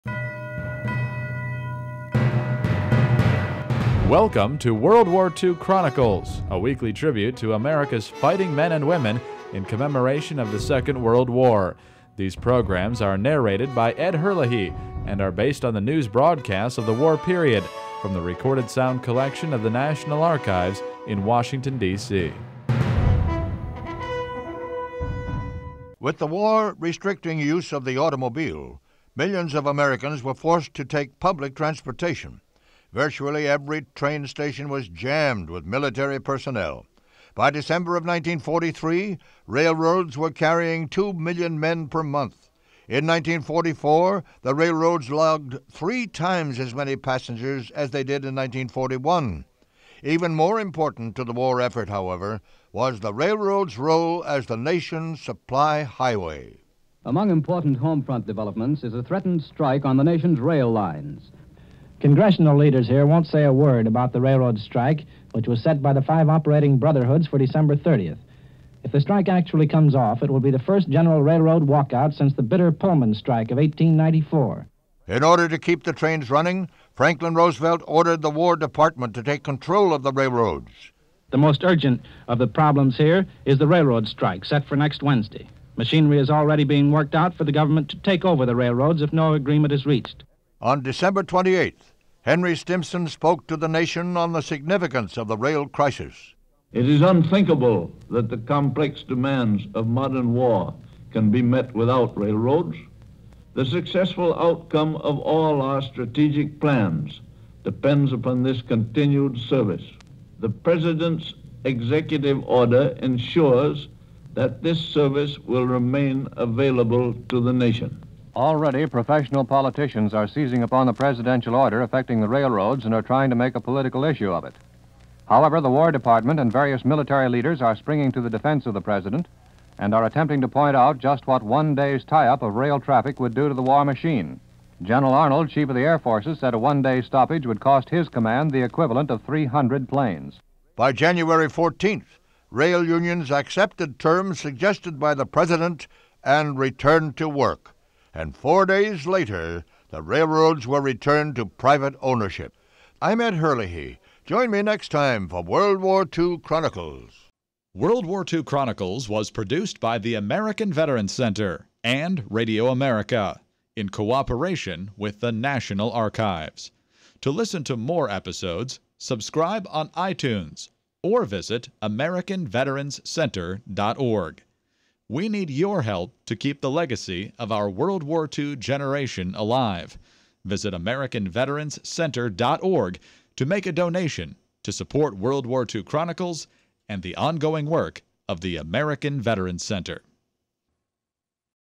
Episode 111: Railroad Strikes
As the war restricted automobile use, many Americans were forced to take public transportation. By December of 1943, railroads were carrying nearly two million people per month, many of which included military personnel. Threatened by a railroad strike, President Roosevelt ordered the War Department to take control of the railroads. By January 14, Railroad workers accepted terms with the government and returned to work.